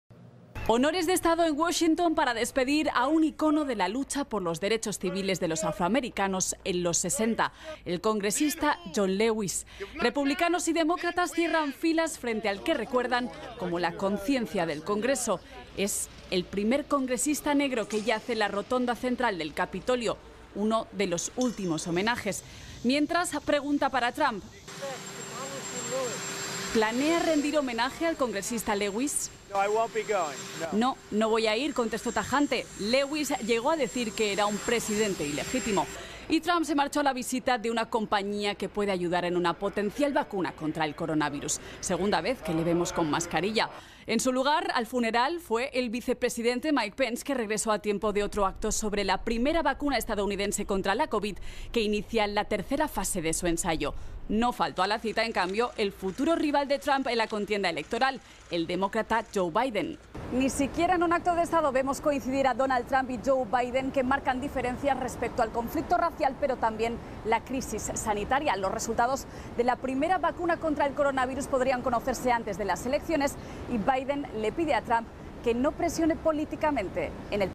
Teleprensa33 | Homenajean a excongresista afroamericano
Estados Unidos hizo un homenaje póstumo al congresista demócrata del Estado de Georgia, John Lewis, reconocido por luchar junto con Martín Luther King y otros líderes por las libertades civiles.